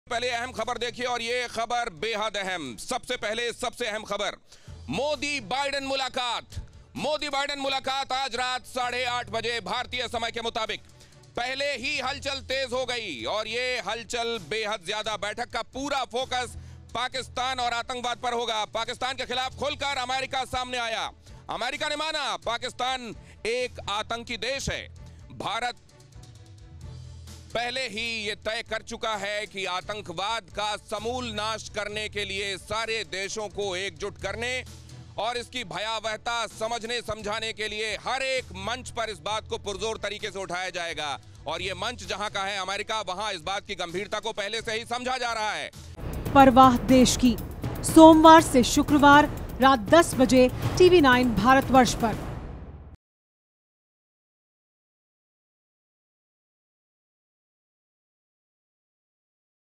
मोदी-बाइडेन की मुलाकात से पहले हलचलें तेज, बैठक का पूरा फोकस होगा पाकिस्तान-आतंकवाद
मोदी-बाइडेन की मुलाकात के बाद हलचलें तेज हो गयी हैं.इस बैठक का पूरा फोकस पाकिस्तान होगा.
Credit